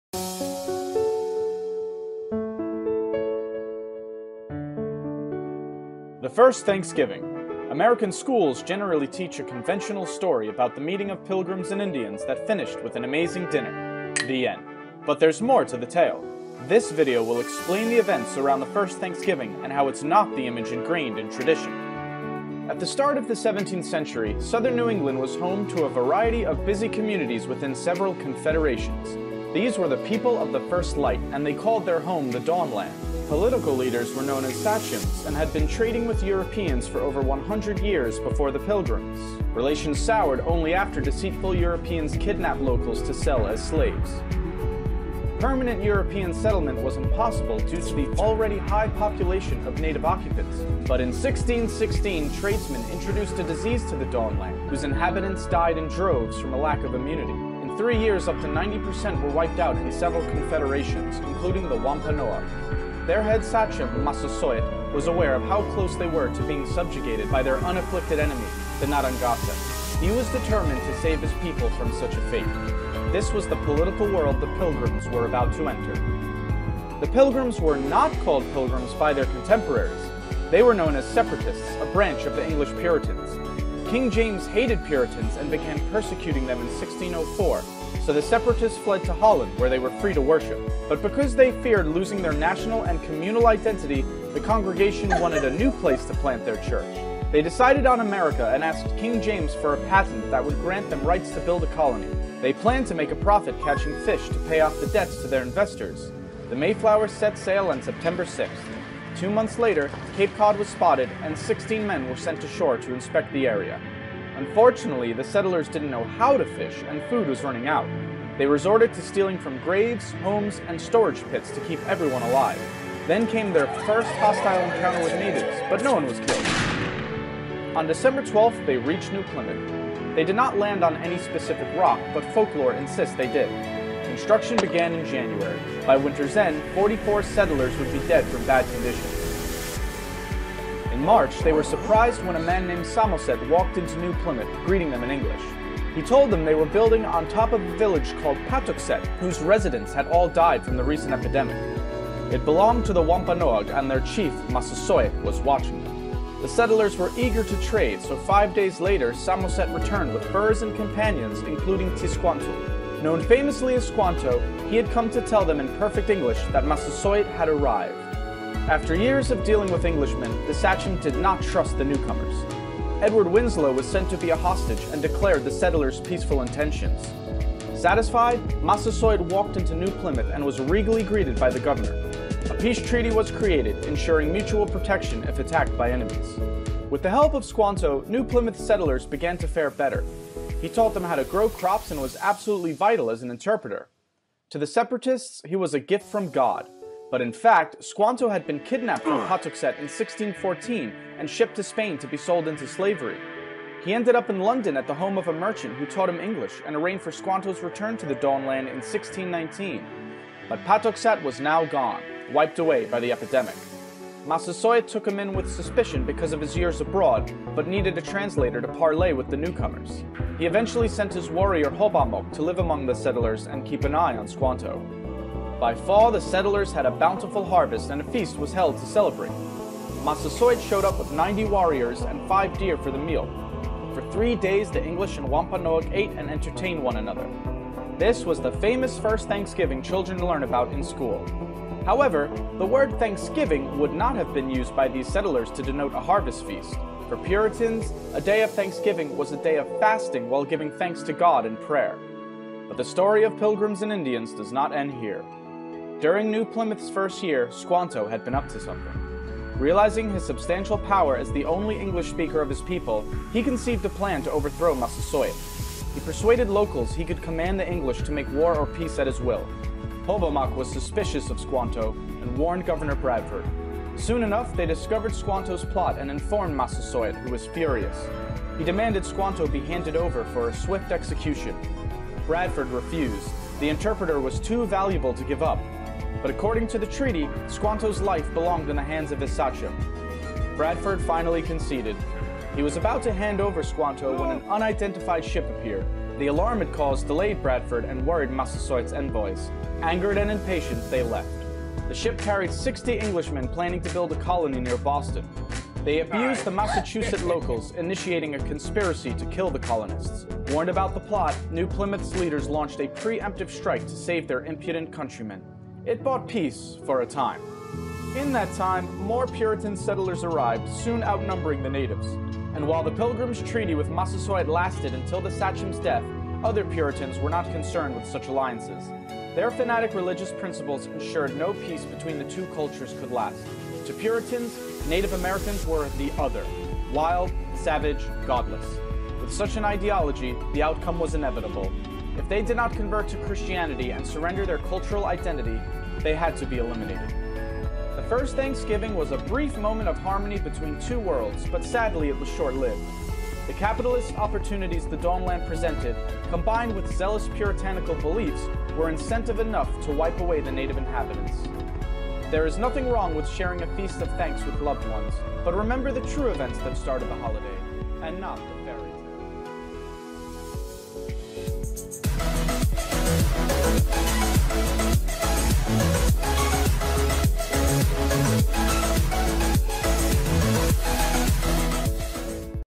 Real History of Thanksgiving
The history of thanksgiving changes with time not just in practice but also in the text books of United States of America.

Knowing the history will enlighten your knowledge.